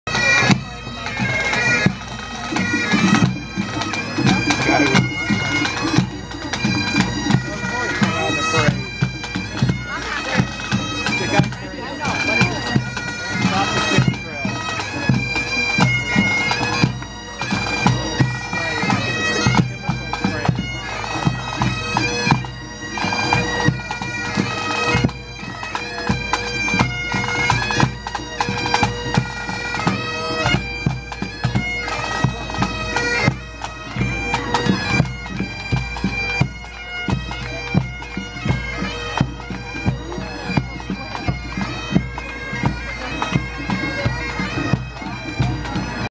Isle of Maui Pipe Band Whale Day 2011
Kihei, Saturday, February 19, 2011, on South Kihei Road for Whale Day.